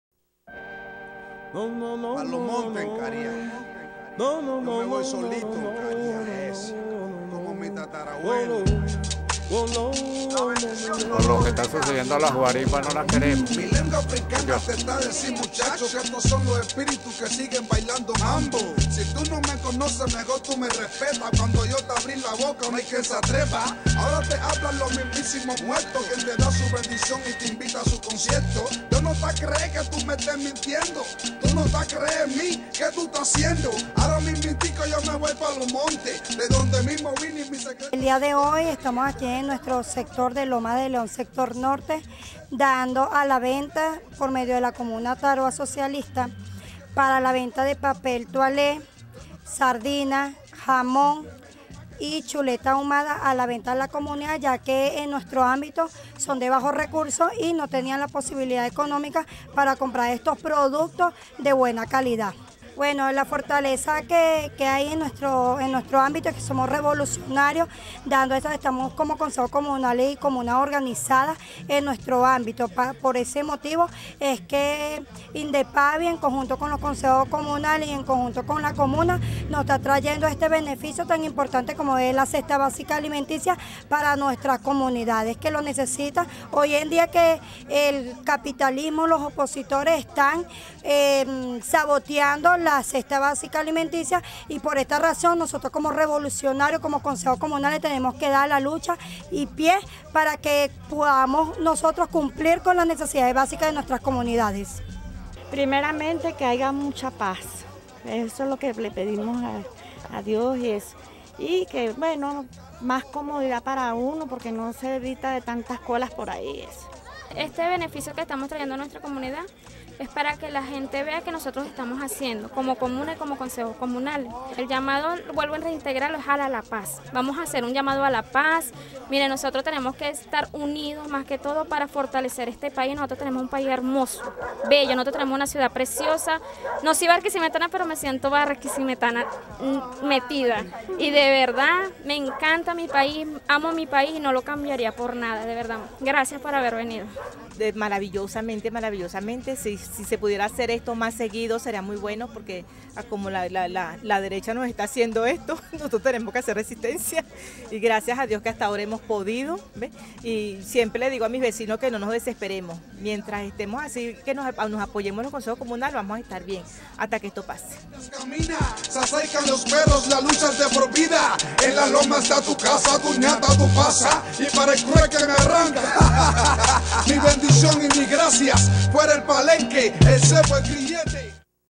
Informativo Comunal: Comuna Ataroa Abastece su Comunidad
REPORTE EN LA CALLE: MOVIMIENTOS POPULARES PRONUNCIA CONTRA FASCISMO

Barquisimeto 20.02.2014
LOS MOVIMIENTOS POPULARES DEL ESTADO LARA PRONUNCIAN CONTRA EL PLAN DEL GOLPE DE ESTADO QUE SE VIENE EJECUTANDO EN VENEZUELA Y HACEN EL LLAMADO PARA CESAR LA VIOLENCIA FASCISTA EN LAS CALLES DE BARQUISIMETO.